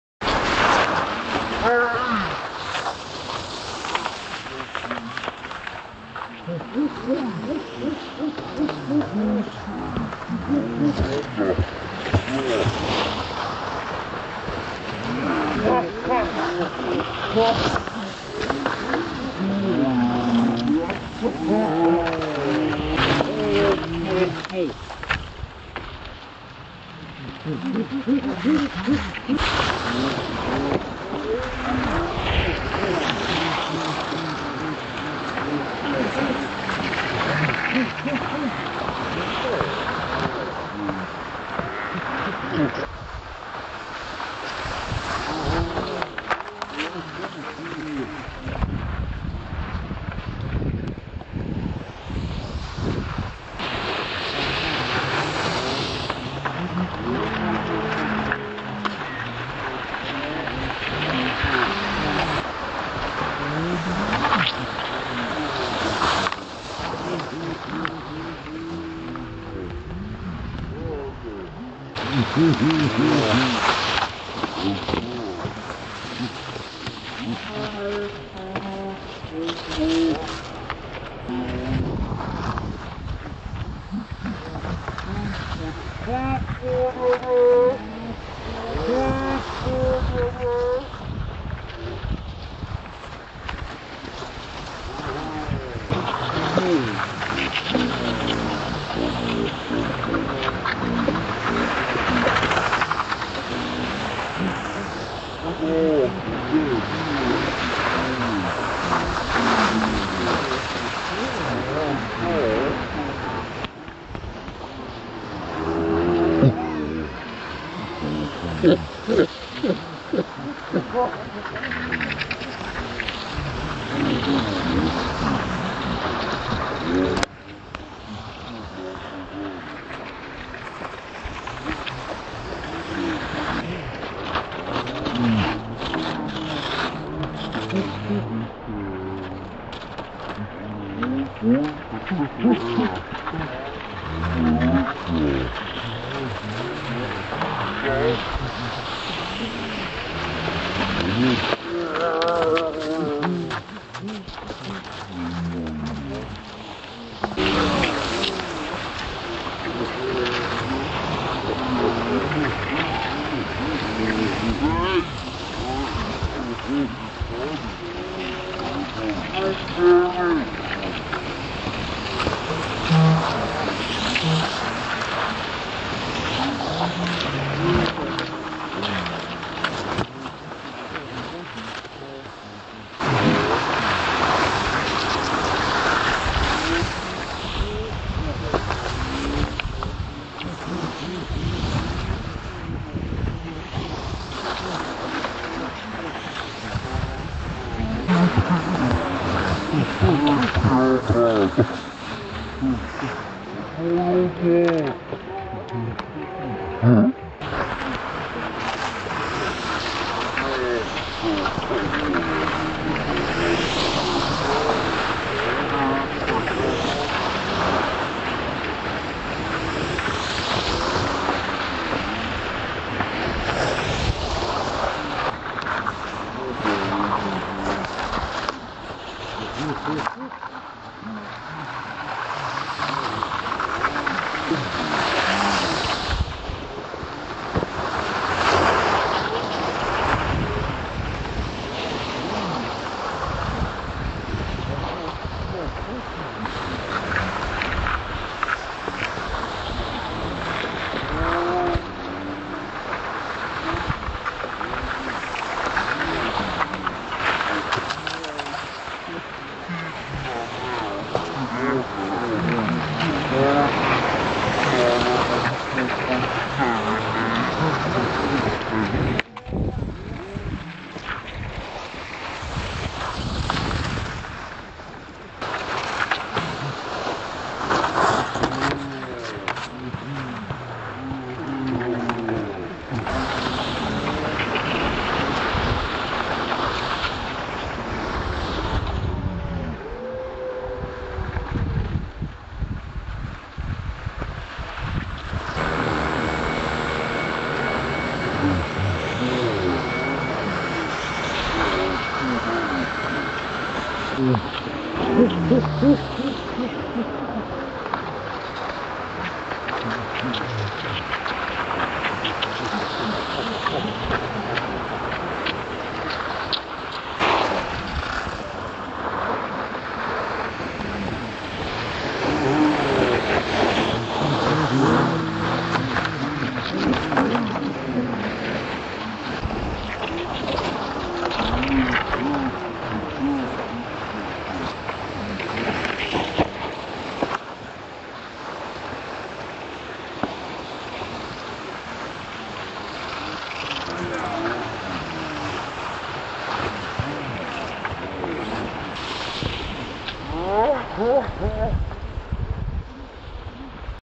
Vasaloppet 2014 crash cut Risberg Slow motion
Ihopklippta vurpor. Slow motion ½ speed.